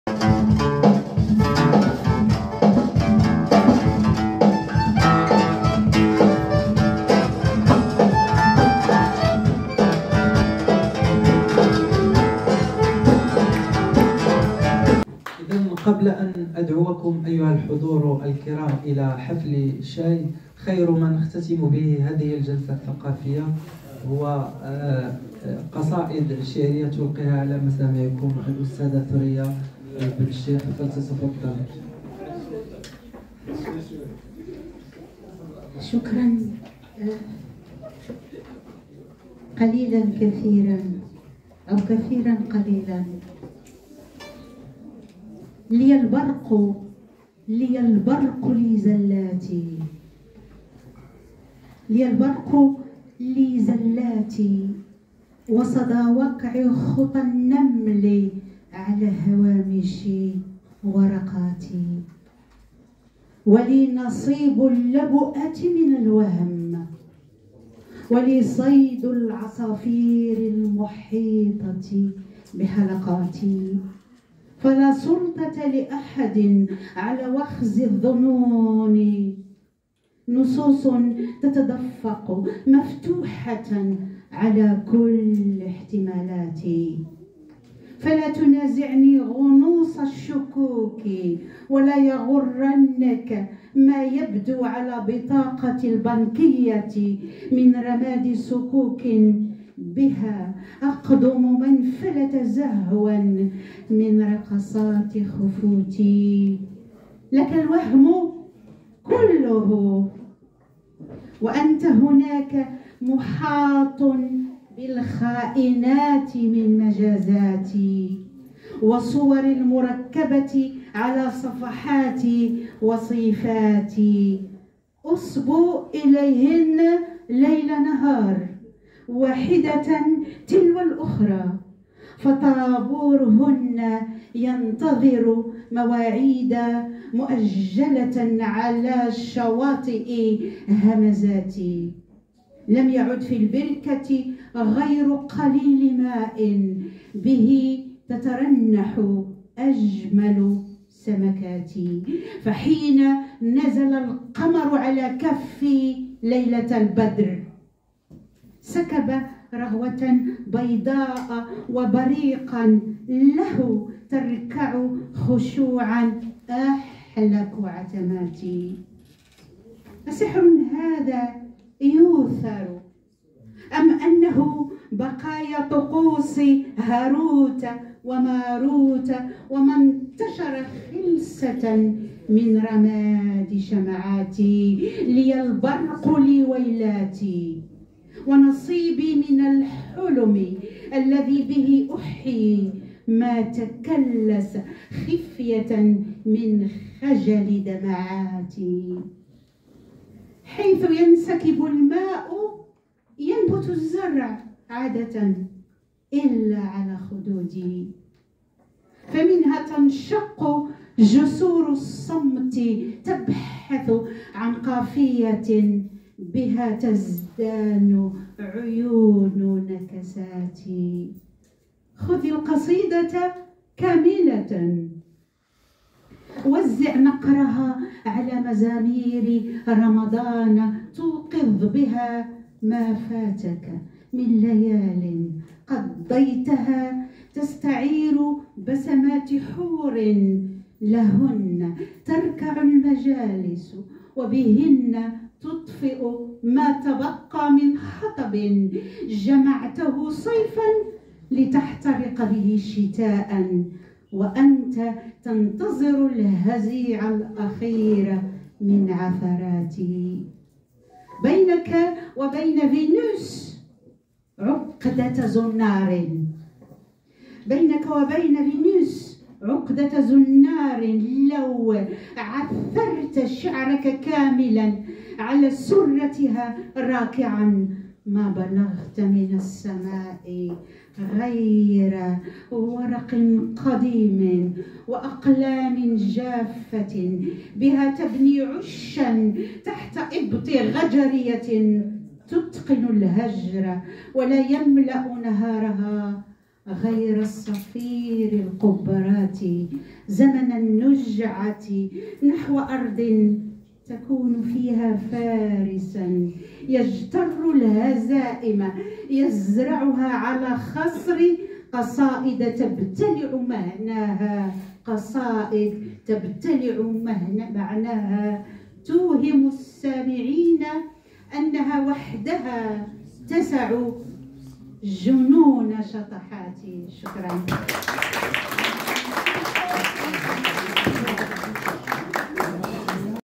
الدكتورة الشاعرة الناقدة ثريا بن الشيخ
قراءة مختارة بمناسبة تقديم وتوقيع مؤلفاتها
بفضاء داركم / مؤسسة الأعمال الإجتماعية للتعليم بالرباط
المملكة المغربية الشريفة .